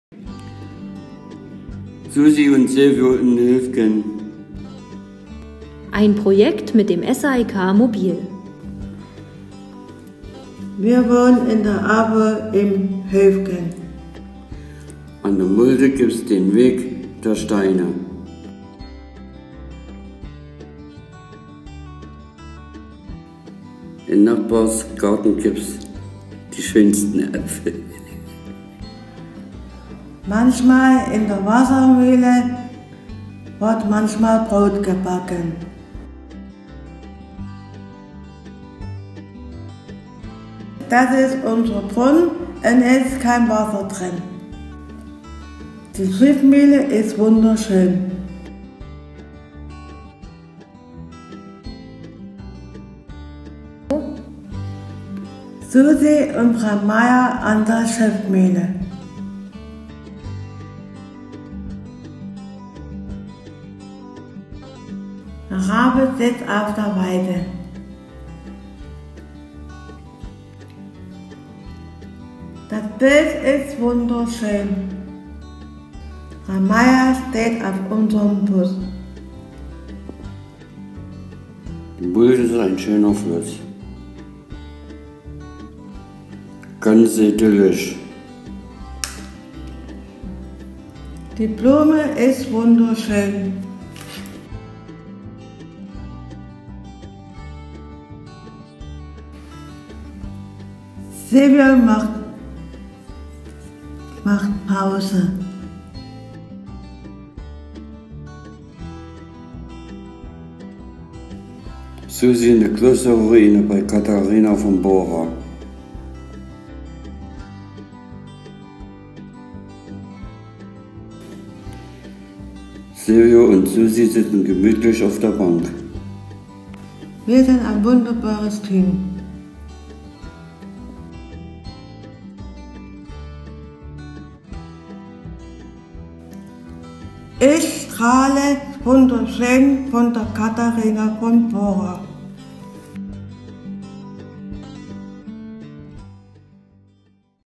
Wo wir leben - ein Fotoprojekt für Menschen mit Behinderung 02 [September 2019]
Fotos mit dem Tablet machen? Für Viele ist das nichts Besonders – für Menschen mit Behinderung aber schon! Vier Tage lang waren wir gemeinsam mit den Bewohner*innen des AWO-Wohnheims in Höfgen bei Grimma unterwegs, um Umwelt, Natur und Ausflugsziele zu dokumentieren. Die Bildgestaltung und Motivauswahl erfolgte durch die Teilnehmenden selbst. Entstanden sind drei wunderbare kleine Fotofilme, in denen die Teilnehmenden ihre ausgewählten Fotos mit der App Adobe Spark Video animiert haben.  Um die Urheber- und Persönlichkeitsrechte zu wahren, wurde manche Inhalte aus den Videos entfernt.

Bahnhofstraße 56
04720 Döbeln

Sächsischen Landesmedienanstalt